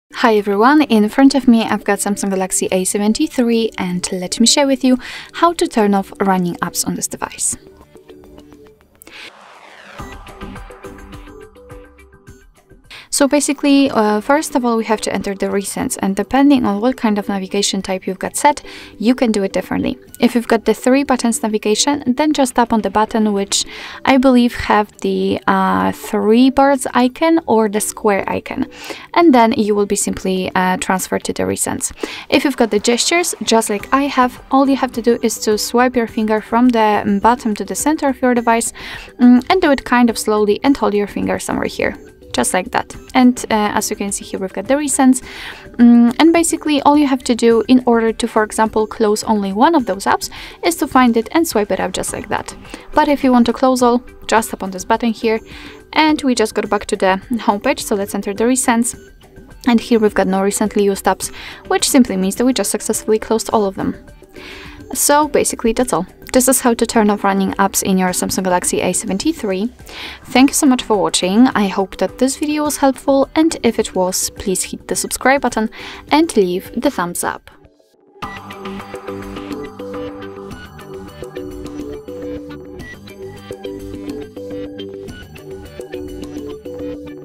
How to Turn Off Running Apps on SAMSUNG Galaxy A73?
In this tutorial video, you will learn how to turn off running apps on your SAMSUNG Galaxy A73 smartphone. Running apps consume system resources and can affect your device's performance and battery life.

"Closing apps on Galaxy A73"
"Force-stopping apps on SAMSUNG Galaxy A73"